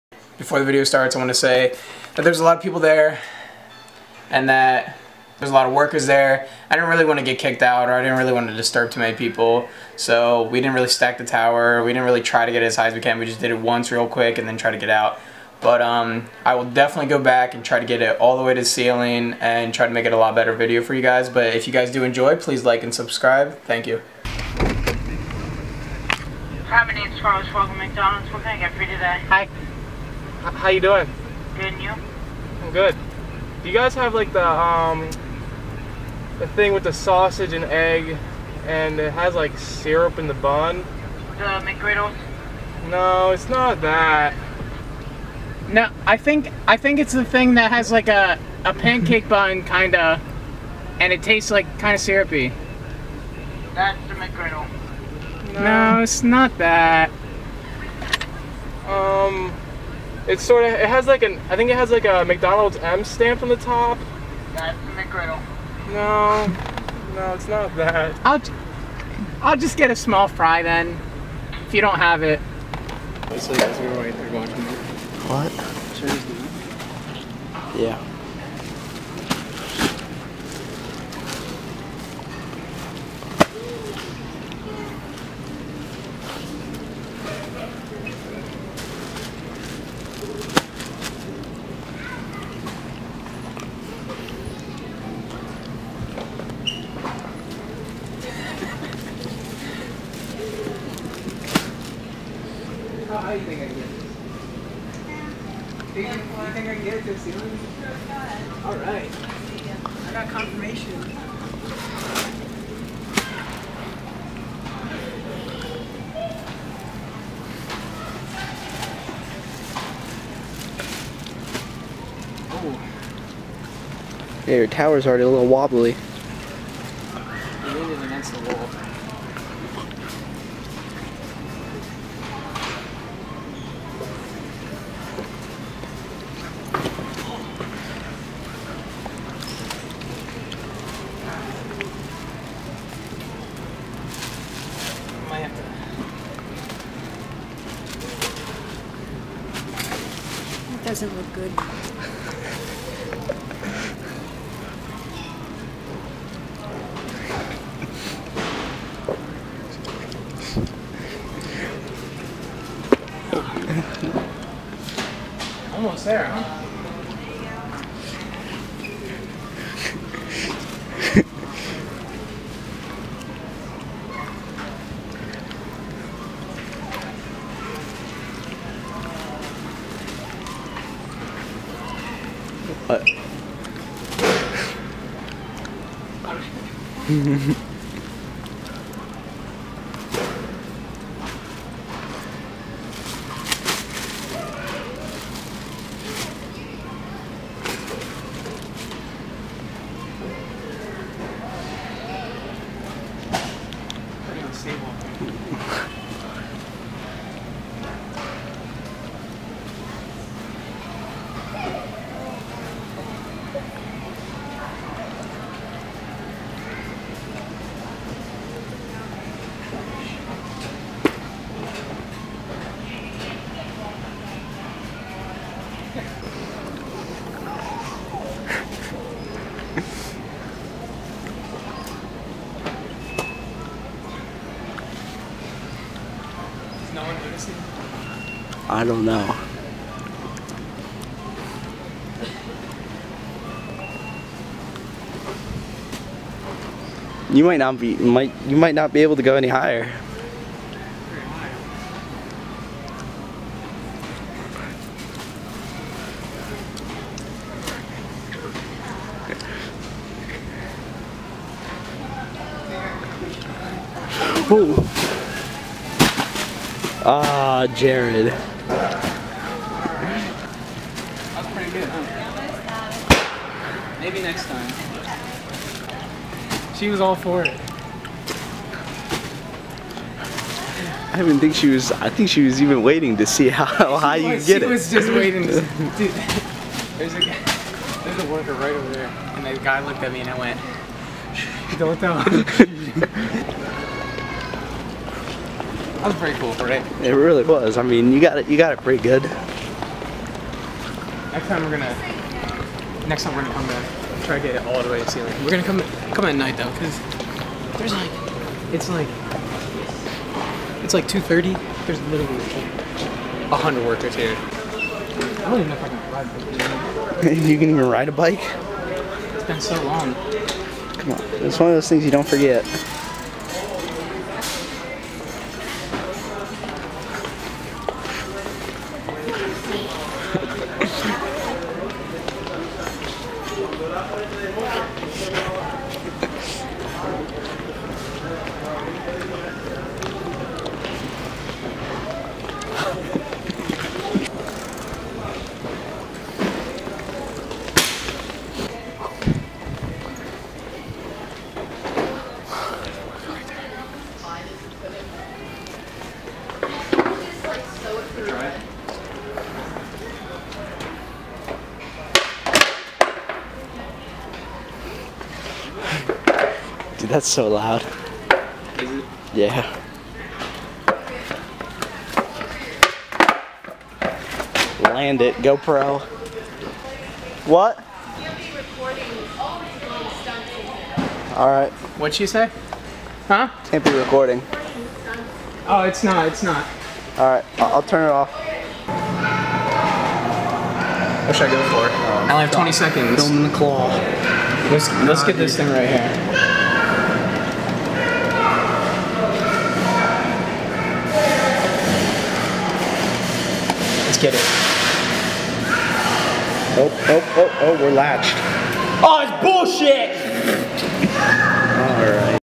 Building toilet paper fort in Walmart
I tried to build a tower out of toilet paper. It was really busy so we only tried once. I will go back again and make sure it gets as high as it can go and make a better video!